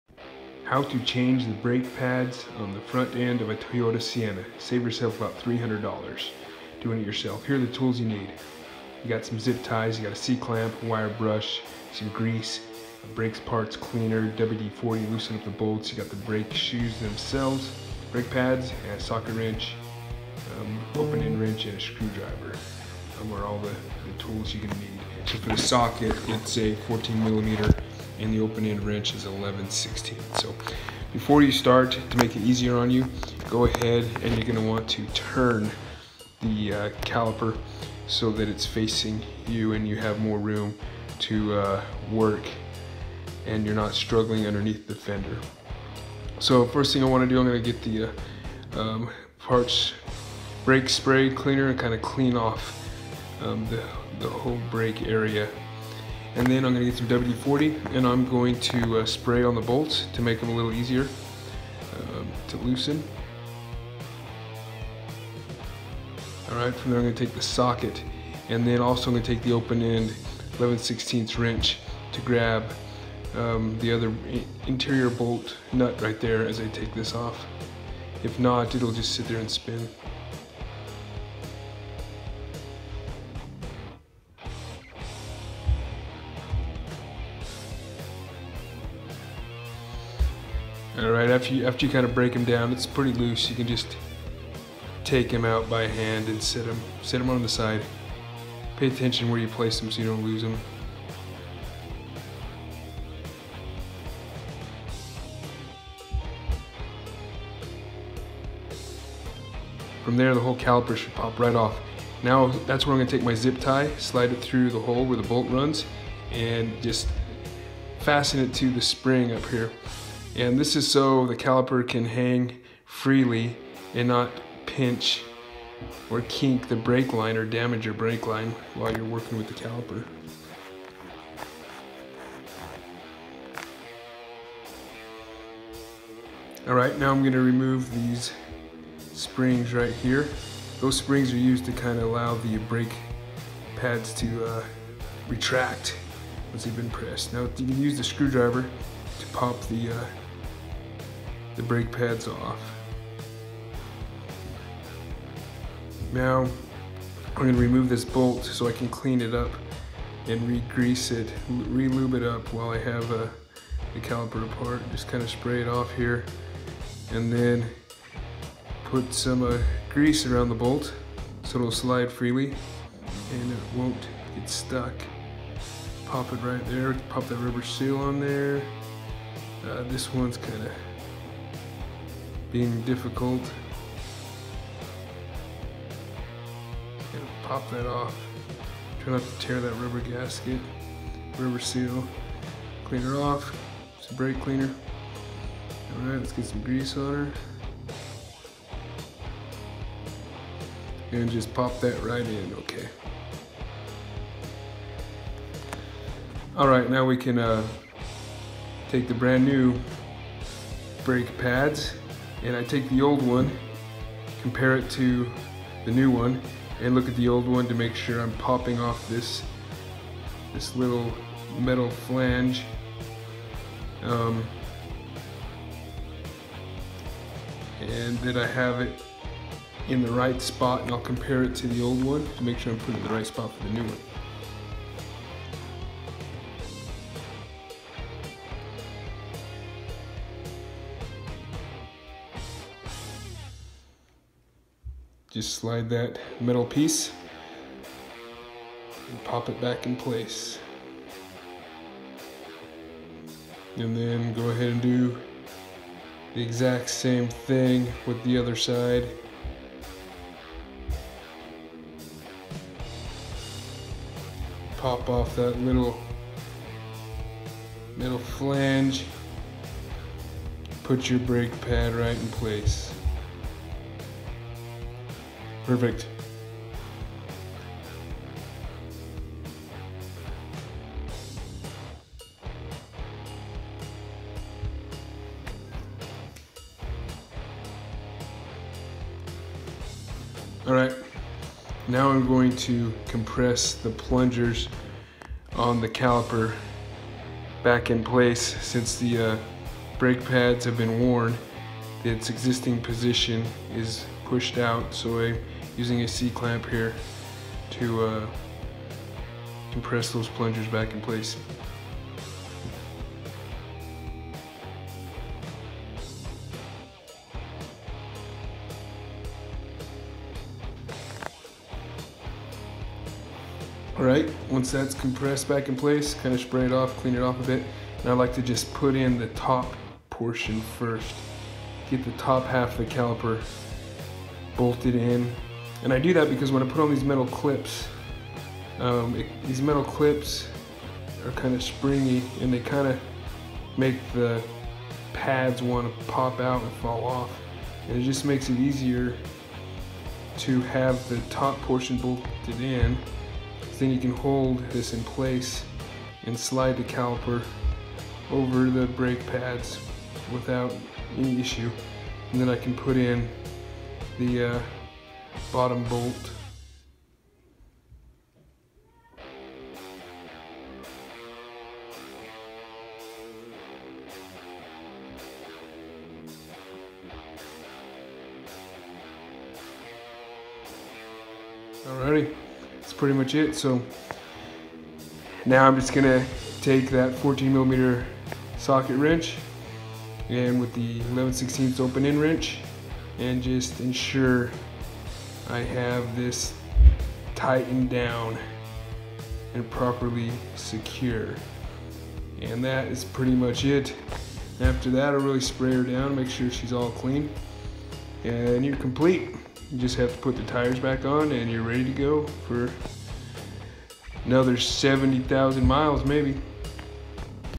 Replacing Front Brake Pads Toyota Sienna 2011-2016
Save yourself easily $300 by changing brake pad yourself.

The average cost for a Toyota Sienna brake pad replacement is between $184 and $301. Labor costs are estimated between $70 and $123 while parts are priced between $114 and $178.